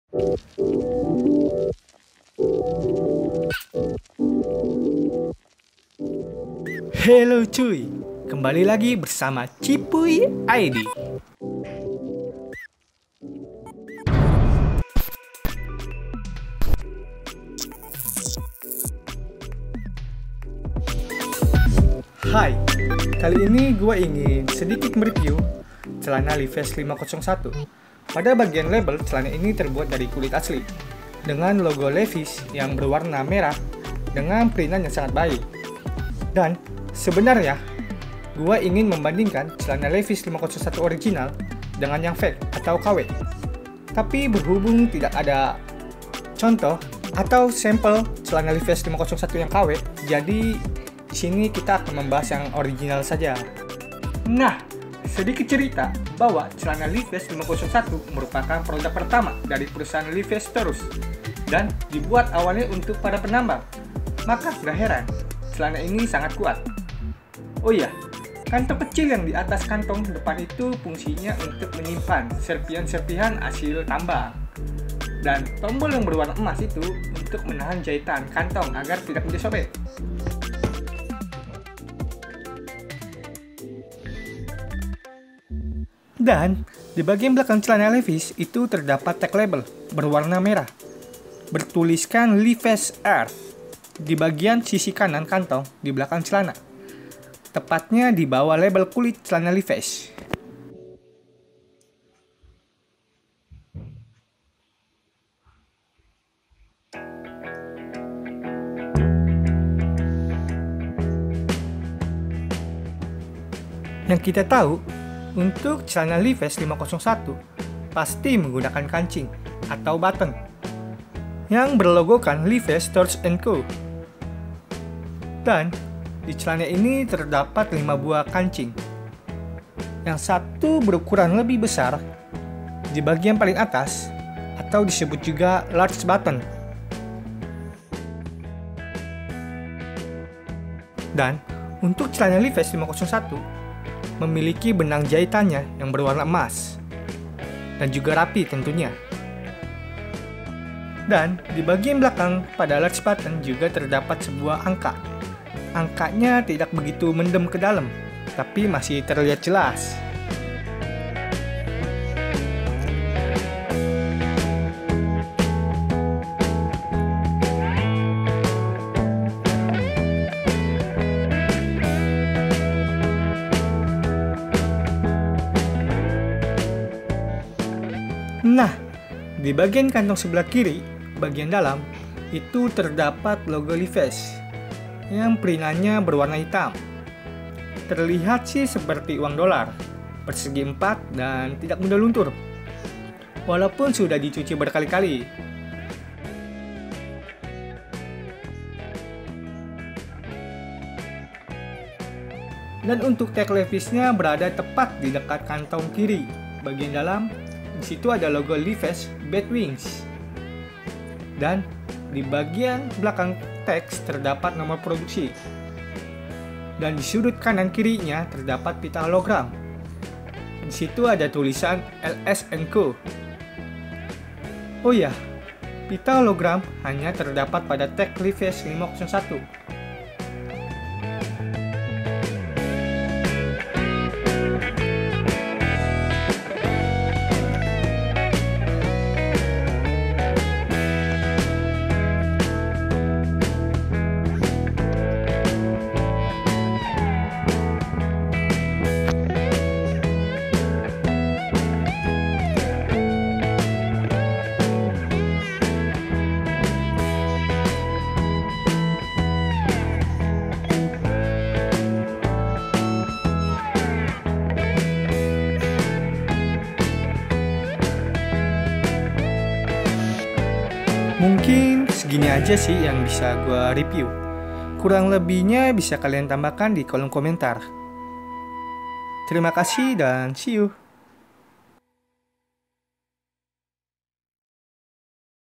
Sedikit Review Levi's 501 Original vs Levi's 501 Asli
Celana Levi's yang awalnya dibuat untuk para penambang kini menjadi celana yang wajib di pakai oleh kalangan pria, apalagi ketika Levi's mempromosikan celananya lewat film coboy amerika. Celana Levis 501 merupakan produk pertama yang di buat perusahaan Levis Strauss & Co. Dan harganya memang relatif lebih mahal dari beberapa celana jeans Levi's pada umumnya. Kisaran harga untuk celana Levi's 501 itu biasanya di atas harga 1.5 jtan kayaknya sudah tidak ada lagi harga dibawah 1jtan untuk celana Levi's 501 ini. Kalau pun ada harus diragukan tuh kualitasnya kecuali second atau mungkin diskon besar"an yang dilakukan oleh official store Levis. Sekarang sudah banyak official Levi's di salah satu jual beli online.